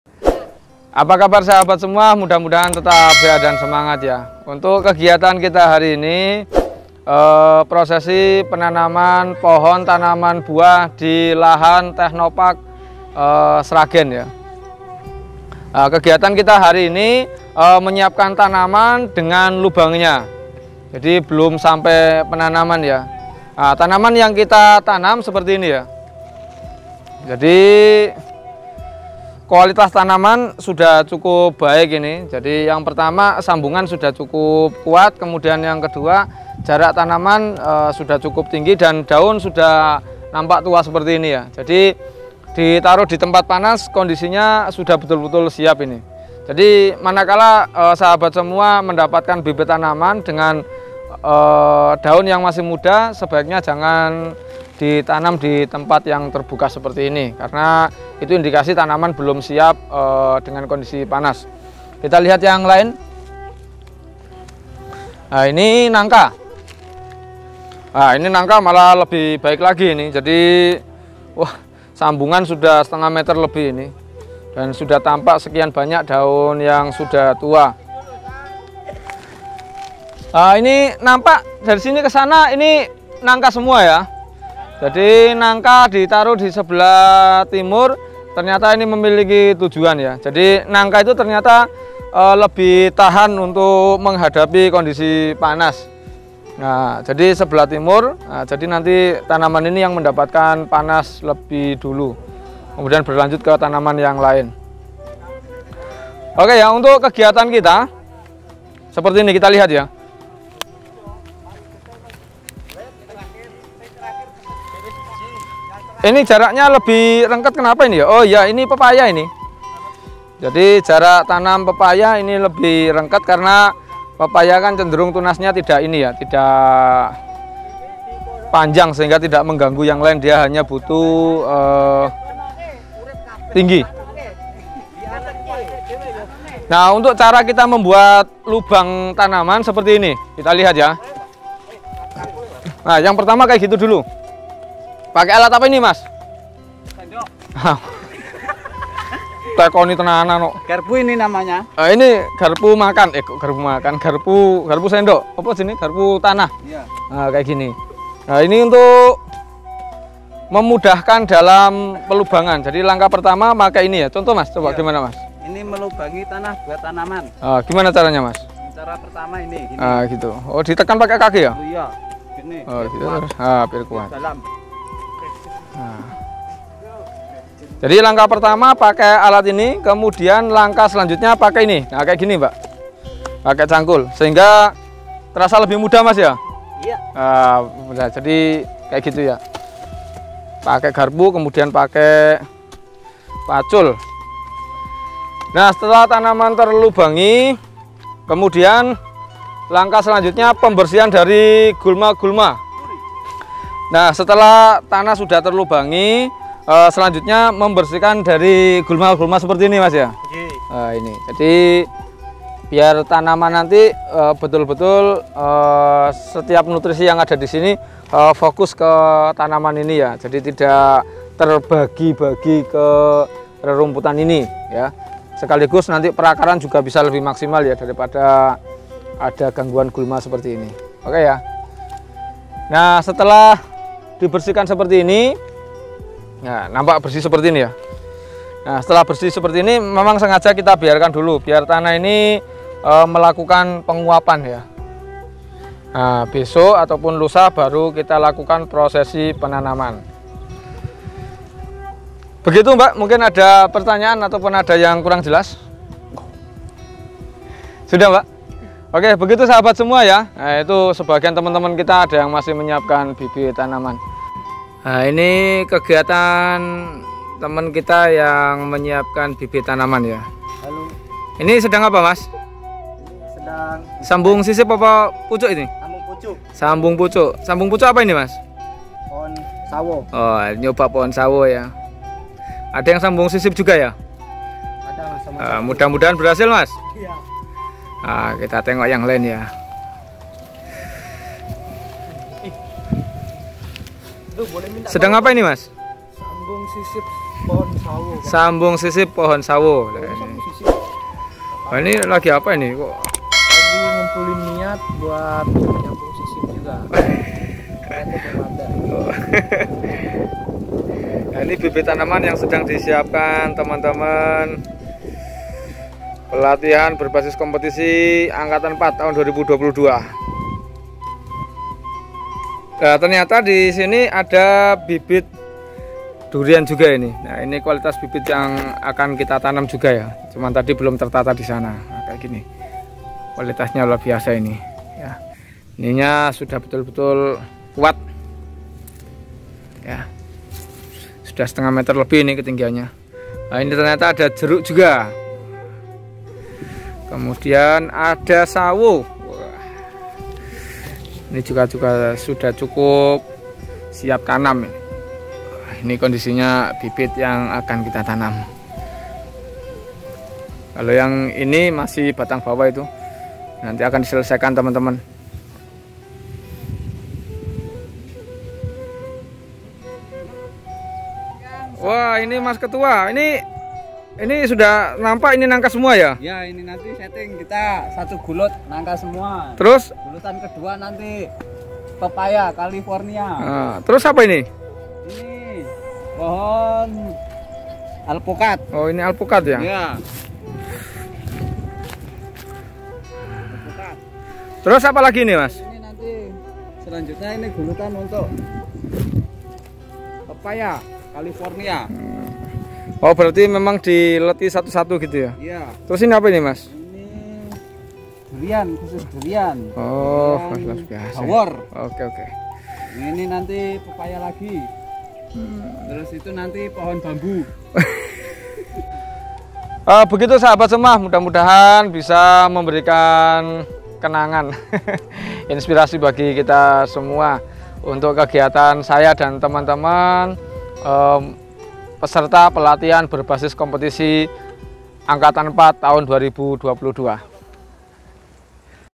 persiapan lahan untuk penanaman bibit aneka buah di technopark sragen
alhamdulillah persiapan penanamam tanaman buah di technopark sragen.
bibit yang di tanam aneka jenis rencananya,
alpukat, nengka, durian, sawo, dan pepaya kalifornia.

semangat untuk teman teman peserta pelatihan berbasis kompetensi angkatan 4 tahun 2022.